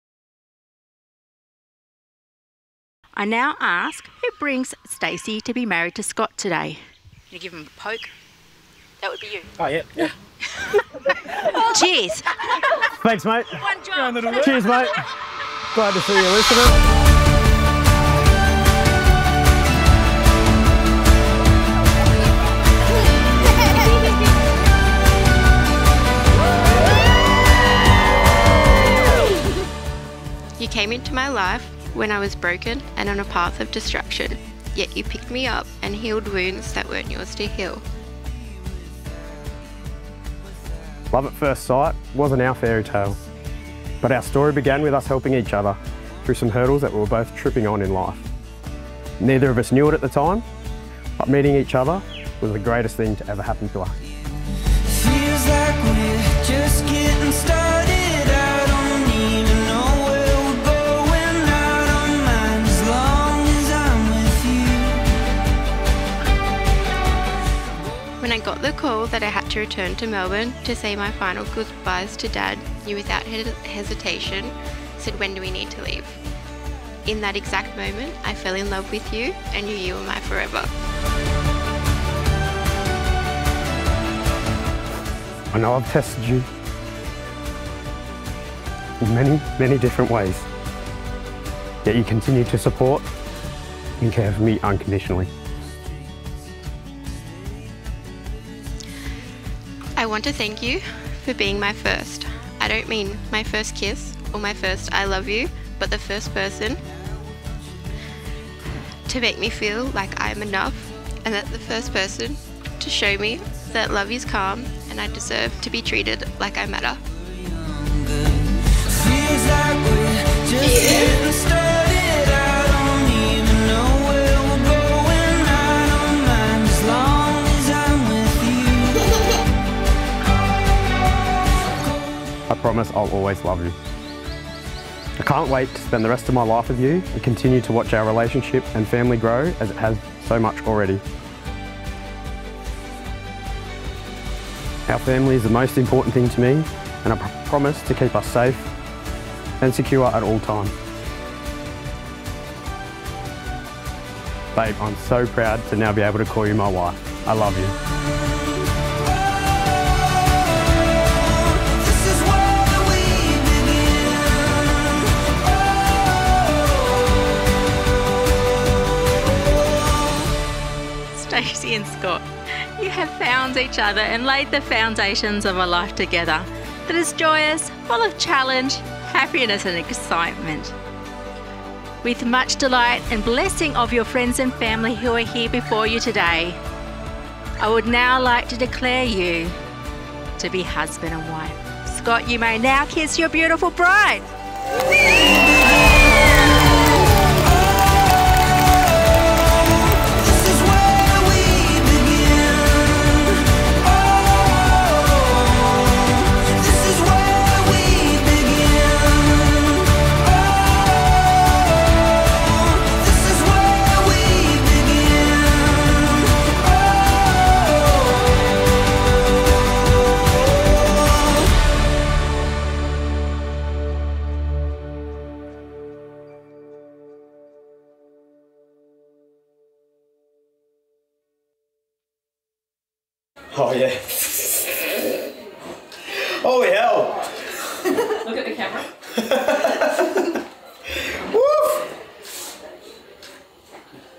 Stacie & Scott - Our Secret Garden Fountaindale
It was a pleasure to capture the beautiful wedding of Stacie & Scott.
MB01FZ2Z3FXVQEY